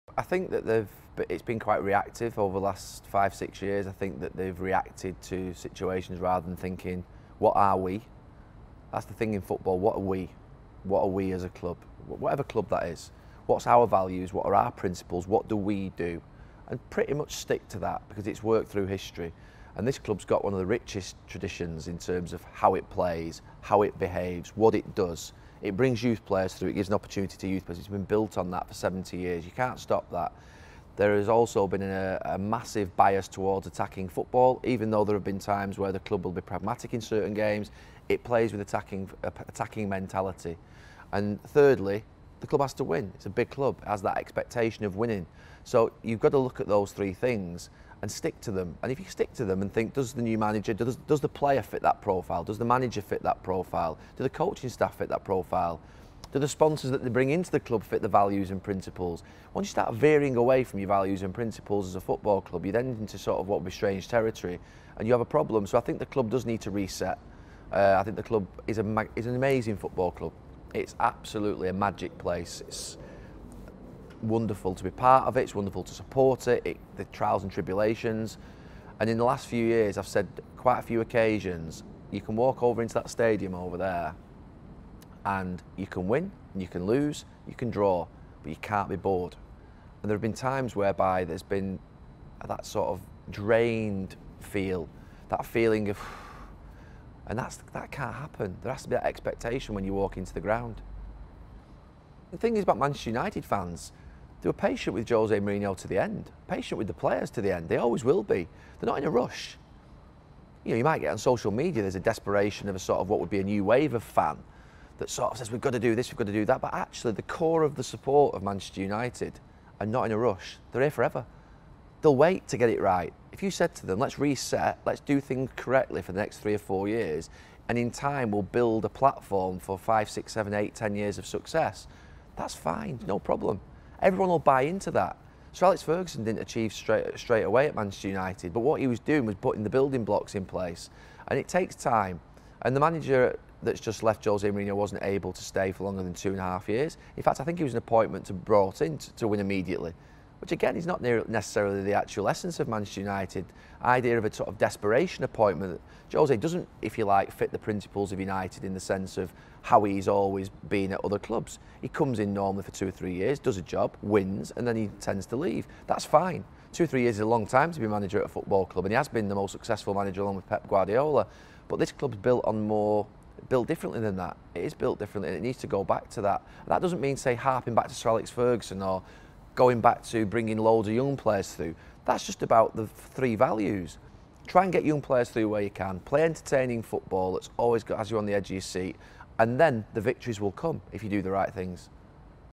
What went wrong for Jose Mourinho at Manchester United? | Gary Neville assesses
Gary Neville discusses the culture of Manchester United, their values, what it means to be a Manchester United fan and what went wrong for Jose Mourinho.

About Super 6:

Introducing our free to play game! Predict six scores to win the £250k jackpot.